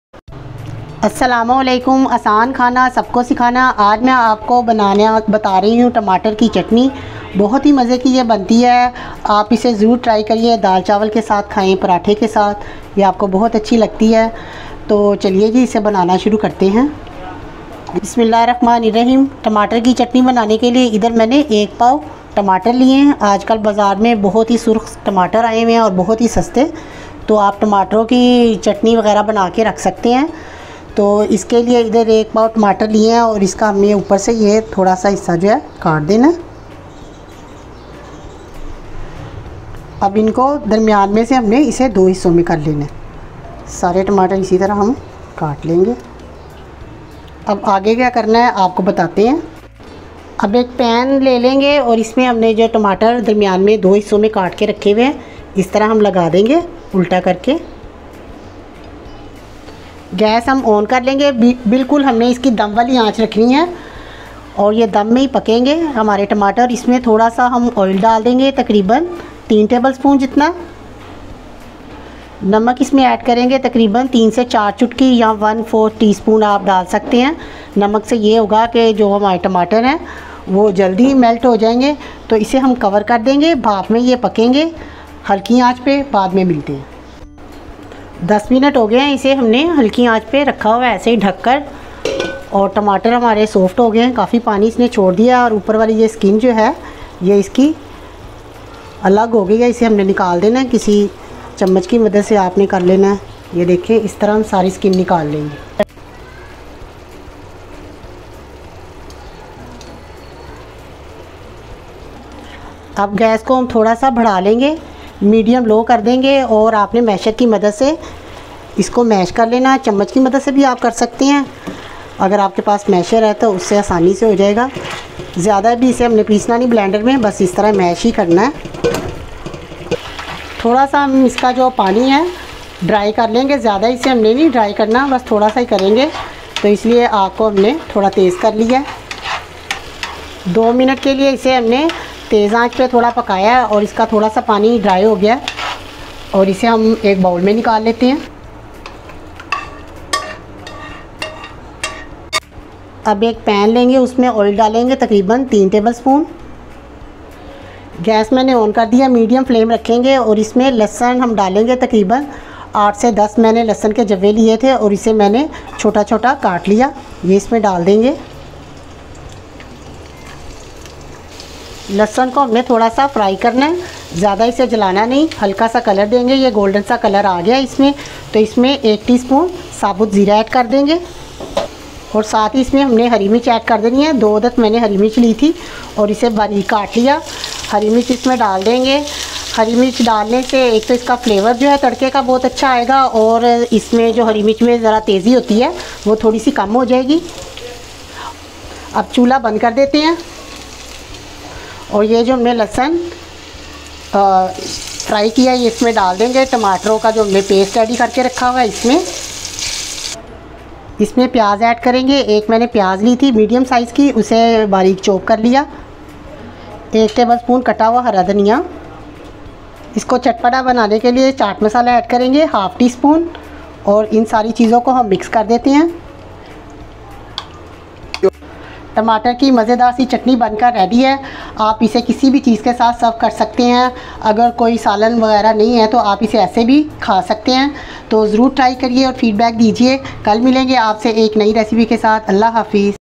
Tamatar Ki Chatni | Tomato Chutney | Tamatar Pyaz Ki Chutney | Golden Kitchen
Tomato Chutney Recipe. Make This Tomato Chutney Once And Enjoy It.  Prepare A Complete Easy And Delicious Tomato Chutney Recipe Step By Step Complete By Golden Kitchen.

Assalam Alaikum Welcome To My Golden Kitchen.

Asan Khana Sab Ko Sikhana,
Aj me ap ko bilkul asan tarike se tamatar ki chatni banane ka tarika batao gi ye bohat he mazedaar or tasty banti hai ap is ko zaroor try karna or muje feedback zaroor dena k kaisi bani ap ki recipe hum pher meli gy ap se aik new recipe k sath, thanks.